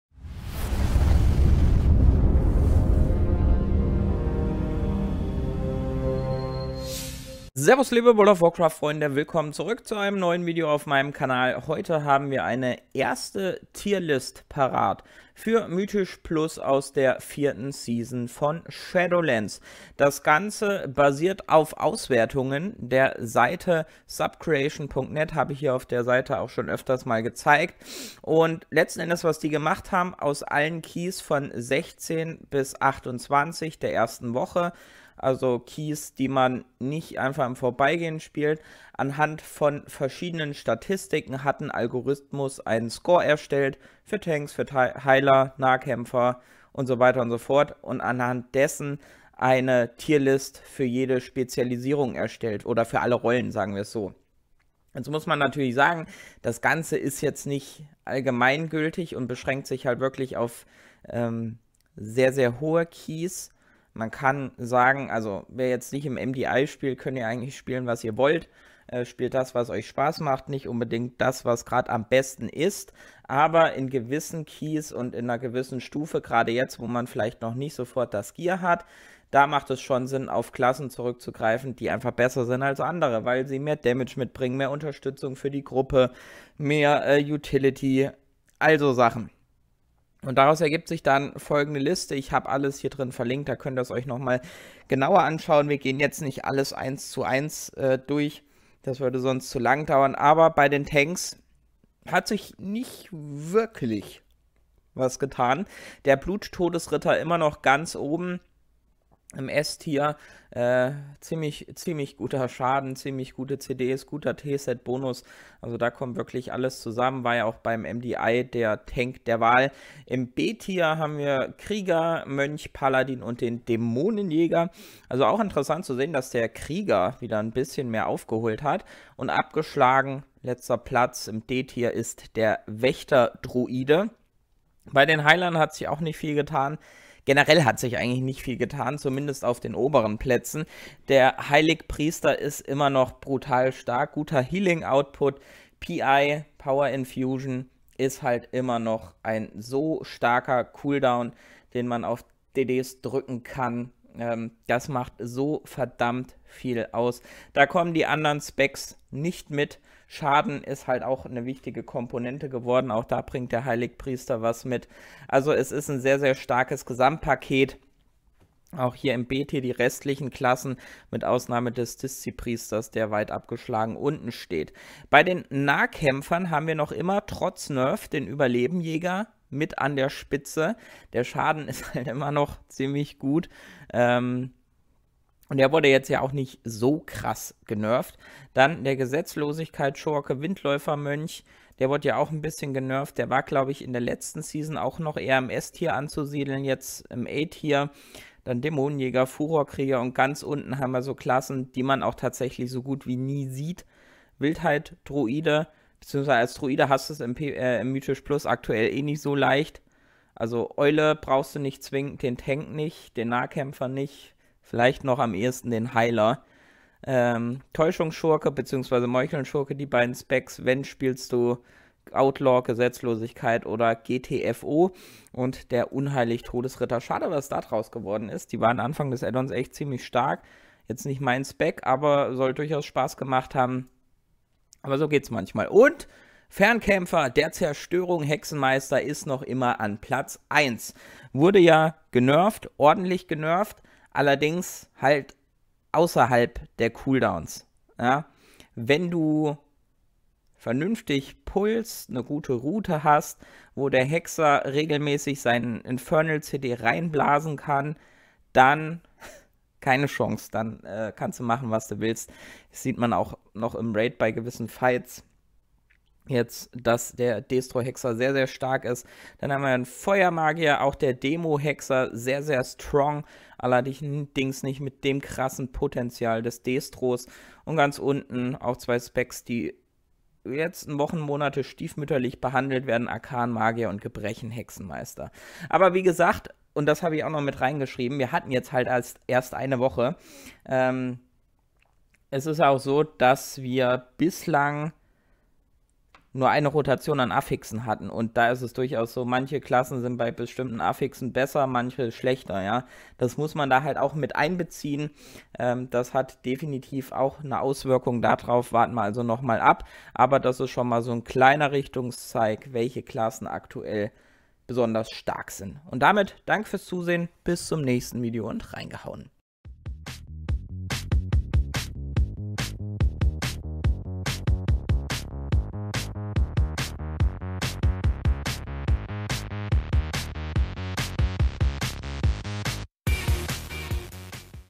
Statistische Auswertung: Die erste Mythisch+ Tierliste der 4. Season von Shadowlands
Subcreation.net hat anhand mehrerer tausend Keys eine Tierliste für Mythisch+ in Season 4 von Shadowlands erstellt.